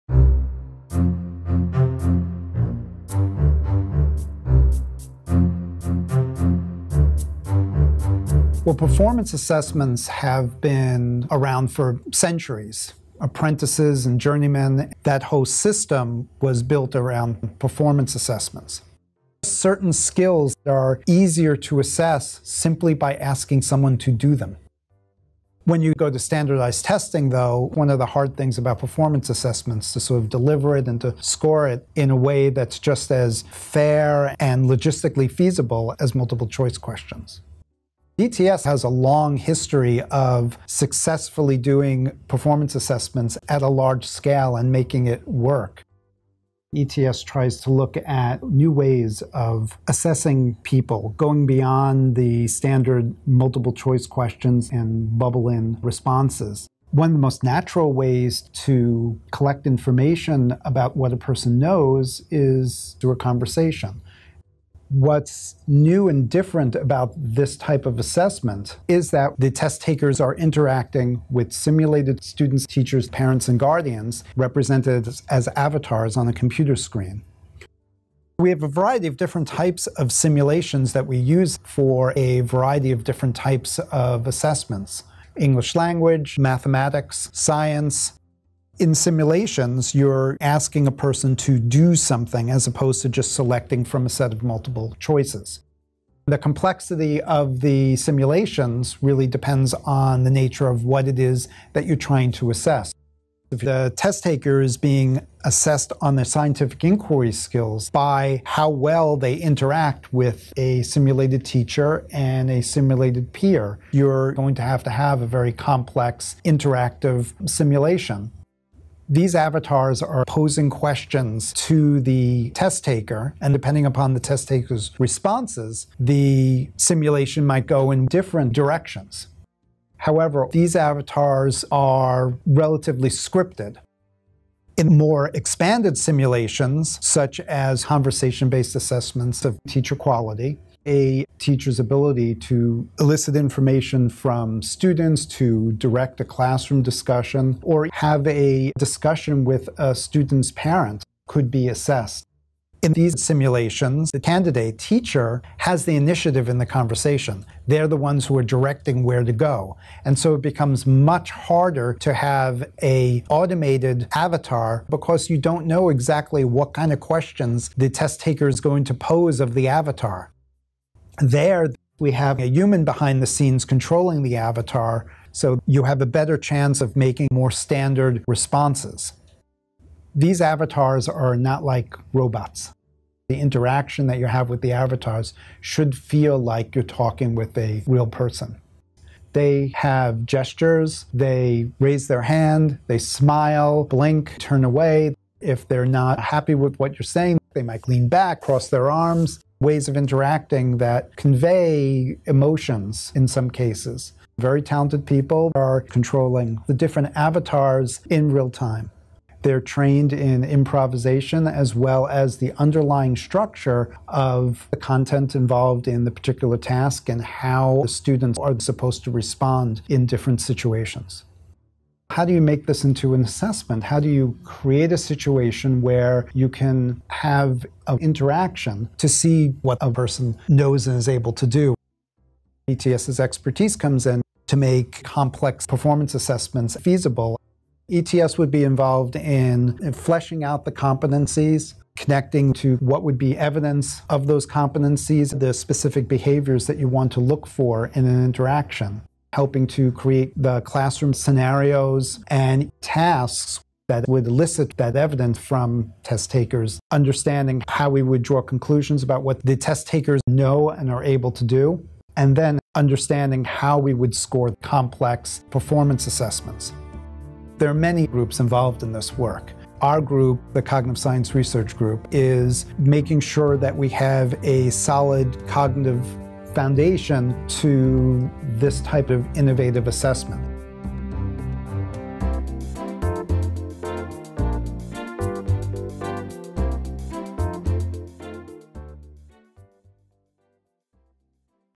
Assessments Using Avatars and Simulations | When Tests Talk Back | ETS
ETS research explains advances in technology and practices make performance assessment more real and scaleable. for more information.